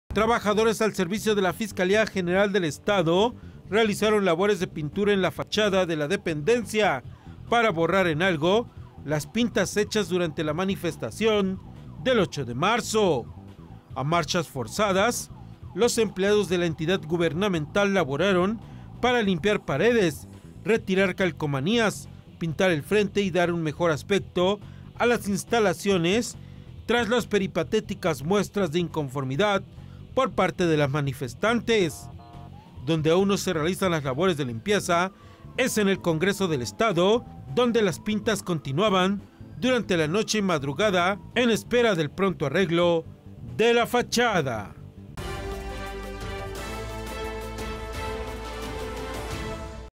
limpian fachadas | Las Noticias Puebla
Trabajadores del estado realizaron labores de pintura en la fachada de la dependencia.